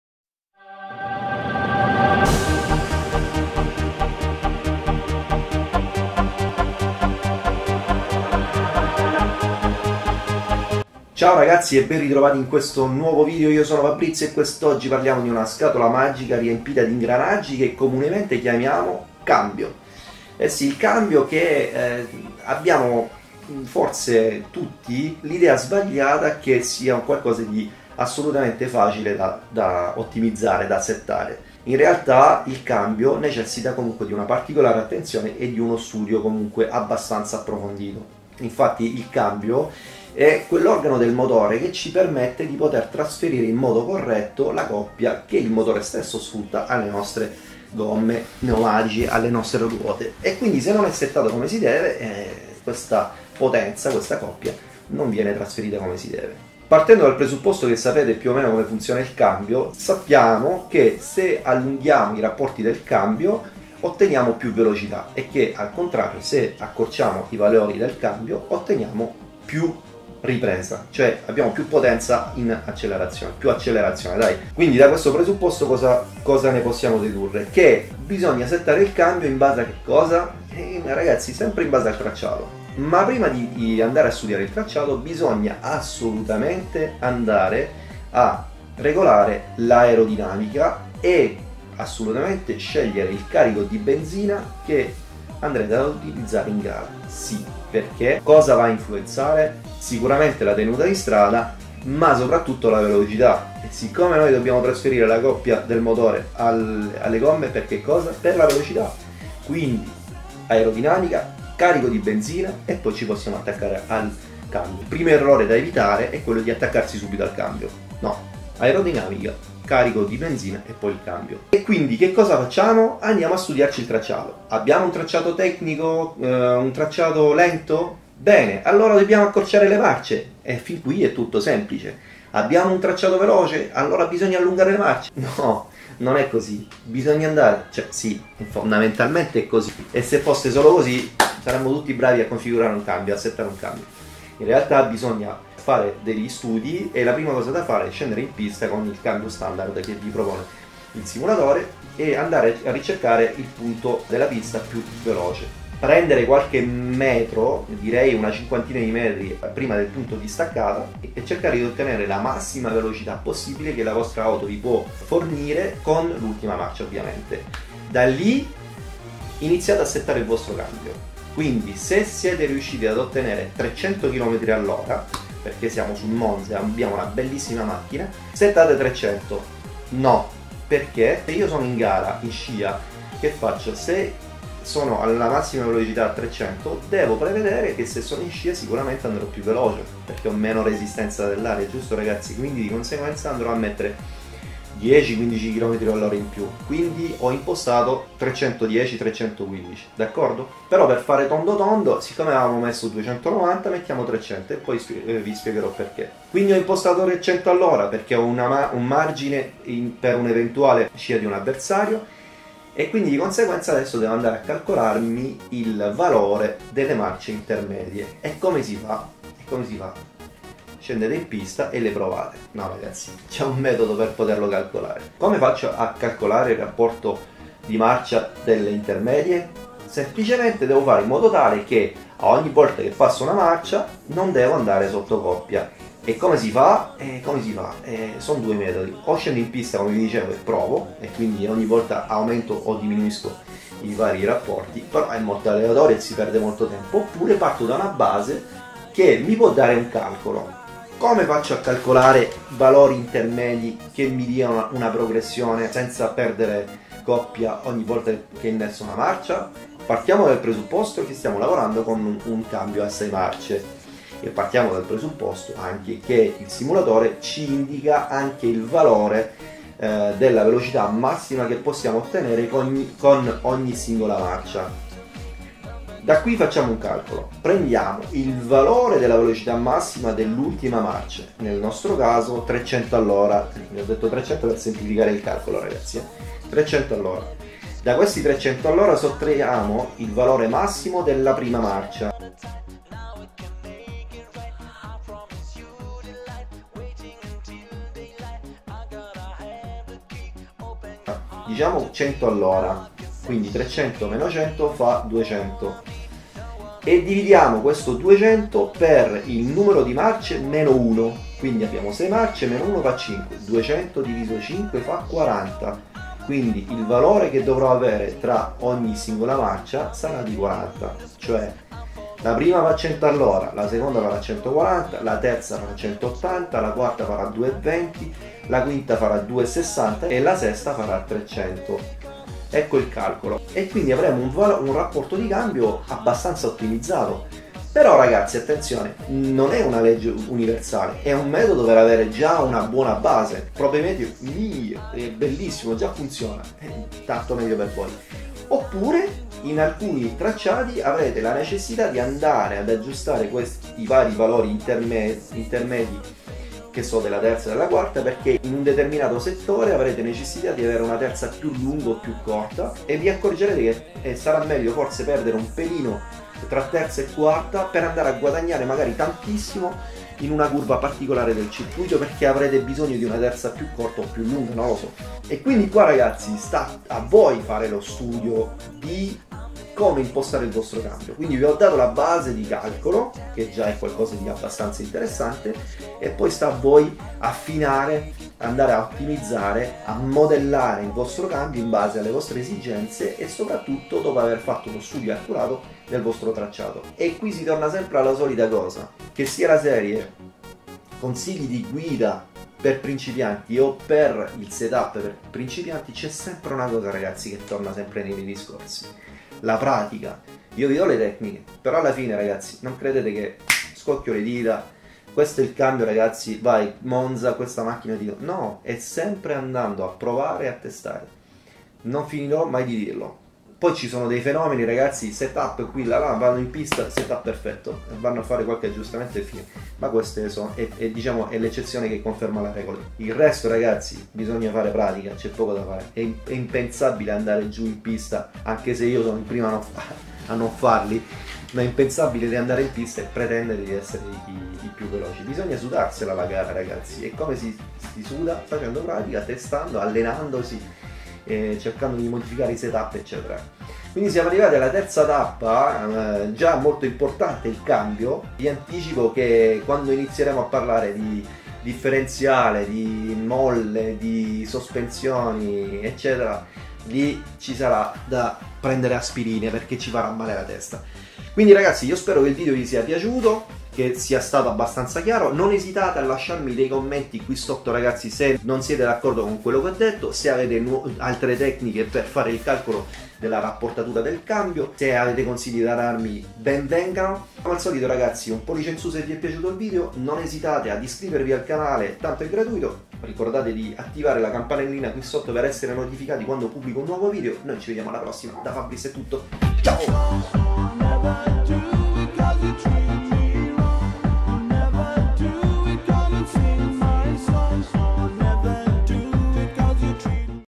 #3. Il Cambio - Il Setup per Principianti [ITA ᴴᴰ]
Terzo episodio di questa simpatica mini serie. Oggi si parla del Cambio! Non esitate a commentare Ragazzi!

► Ricorda!

Buona visione!

► SETUP:
HTC VIVE
Watercooling
Corsair 750W